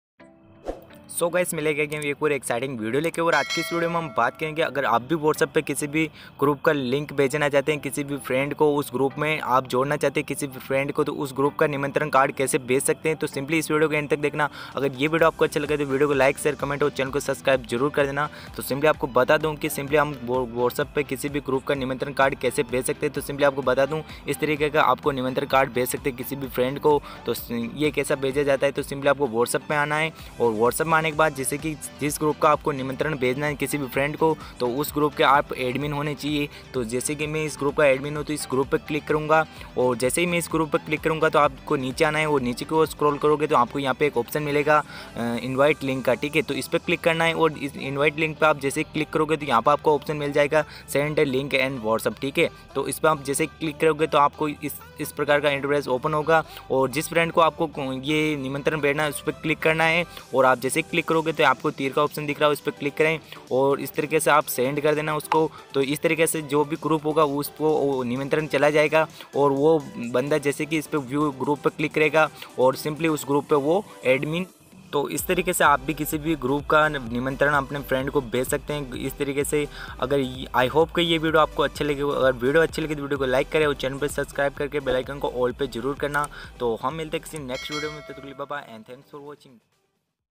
WhatsApp per group ka link share karna seekho || व्हाट्सएप पर ग्रुप का लिंक कैसे भेजें
नमस्कार दोस्तों
इस वीडियो में मैं आप लोगों को बताऊंगा कि व्हाट्सएप पर किसी भी ग्रुप का लिंक कैसे भेज सकते हैं किसी भी ग्रुप या फ्रेंड को ।
किसी फ्रेंड को ग्रुप में कैसे जोड़ सकते हैं ग्रुप निमंत्रण भेज करें ❓

tech with shani
ग्रुप लिंक कैसे भेजें व्हाट्सएप पर